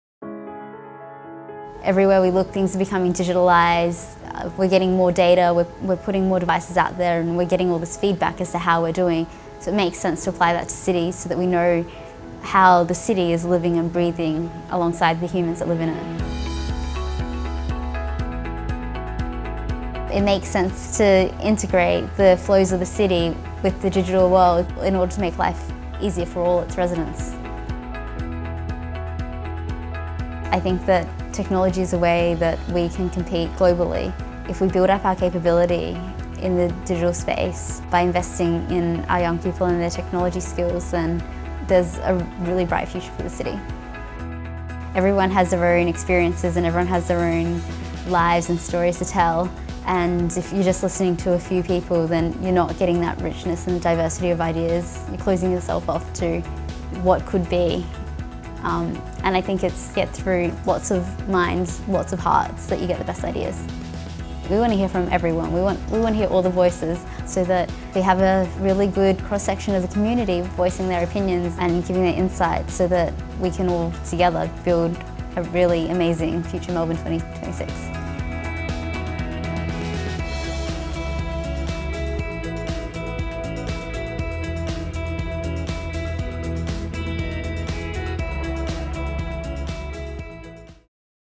Future Melbourne ambassador, Marita Cheng | City of Melbourne
Hear Marita Cheng talk about her role as a Future Melbourne ambassador and the role the community can play in helping build the city’s digital capability.